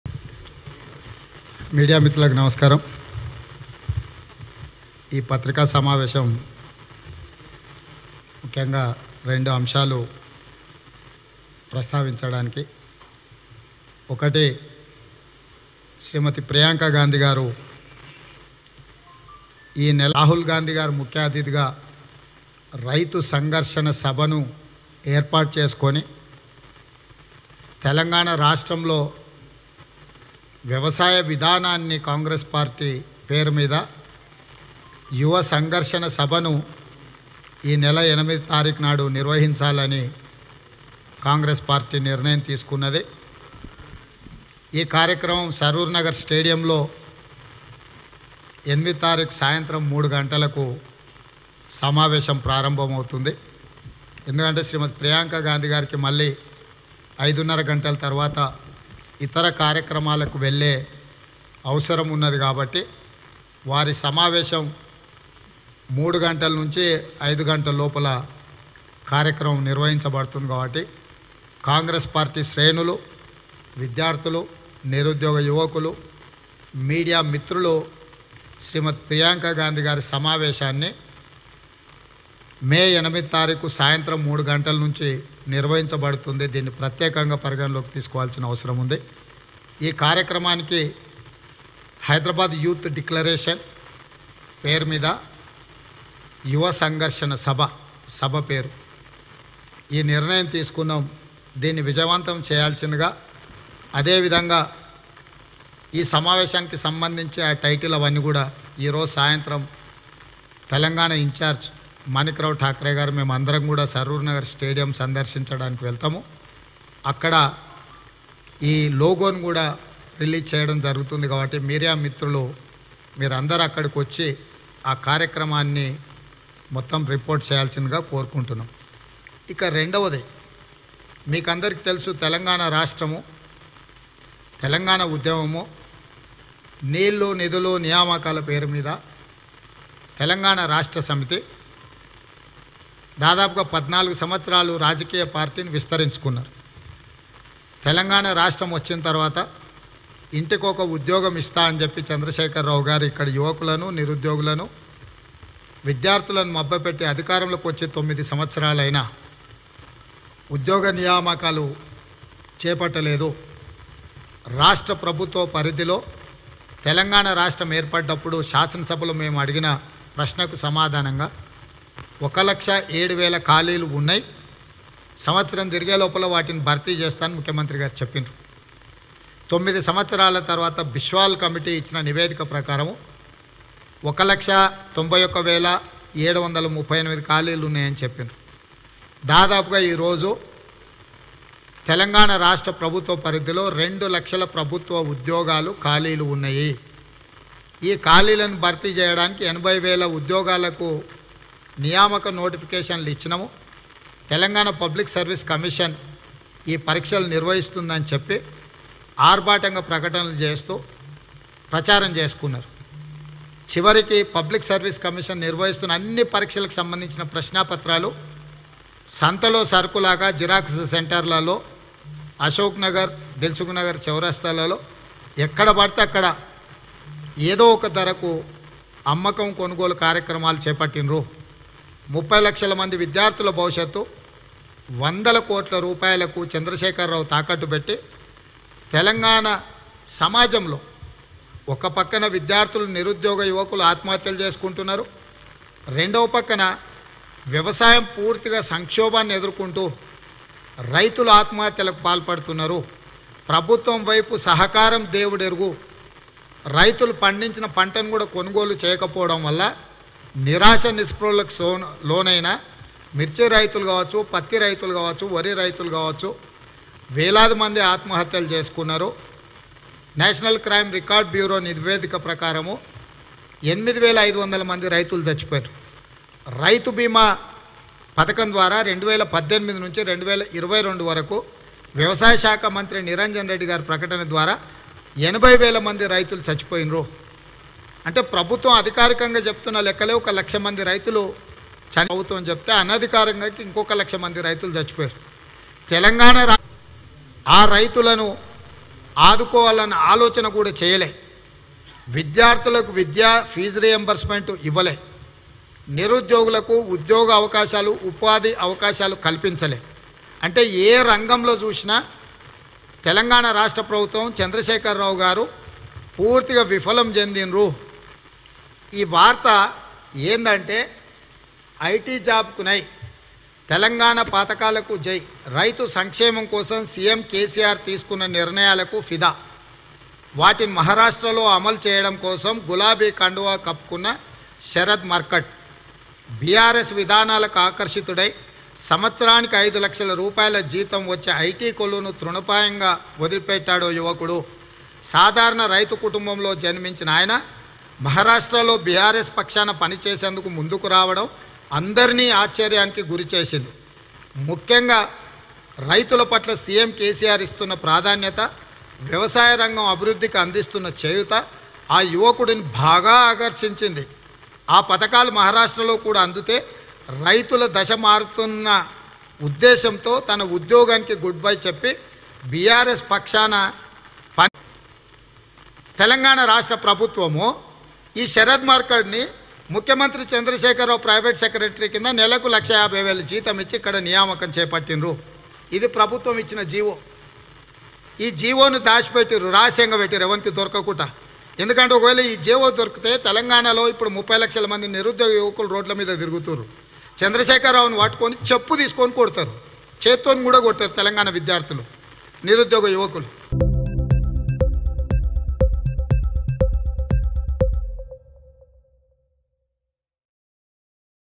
Revanth Reddy Calls Congress Activists For Priyanka Gandhi Saroornagar Meeting | Mango News
Revanth Reddy Calls Congress Activists For Priyanka Gandhi Saroornagar Meeting.